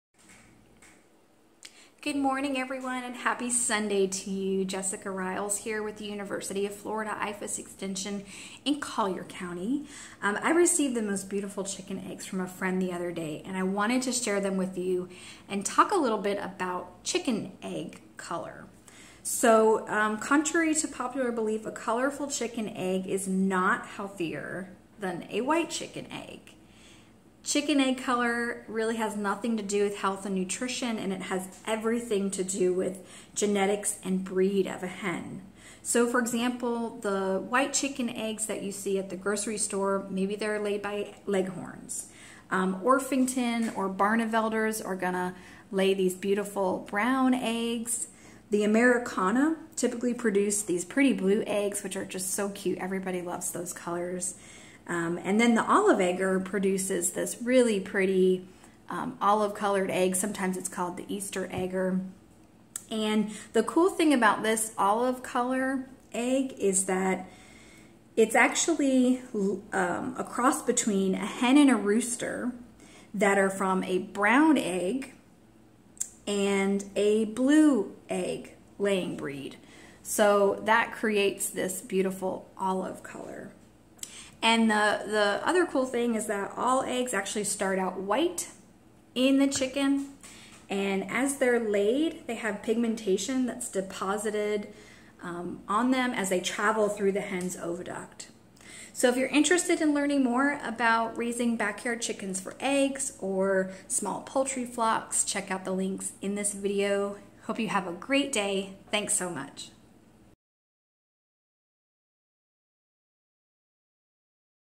Are Colorful Chicken Eggs Healthier than White Eggs?
The color of an egg shell has influence on people's buying choices. Some believe that brown, green, blue, or pink eggs are healthier than white chicken eggs. Learn the truth to the myth!